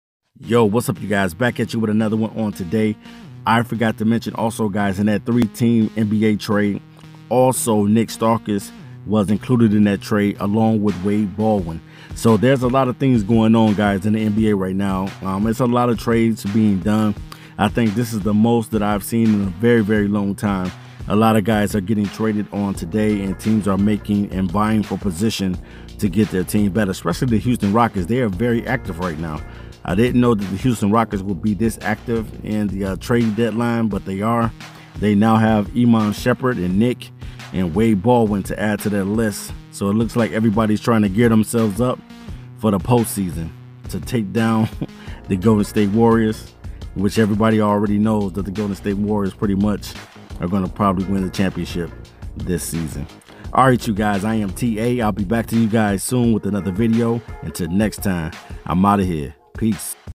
NIK STAUSKAS AND WADE BALDWIN ARE HEADED TO HOUSTON ROCKETS AS PART OF THE 3 TEAM TRADE!!!
Also guys it looks like Nik Stauskas and Wade Baldwin will be headed to the Houston Rockets as part of the 3 Team Trade between Cleveland,Houston and Sacramento Kings..

WE TALK SPORTS PERIOD!!!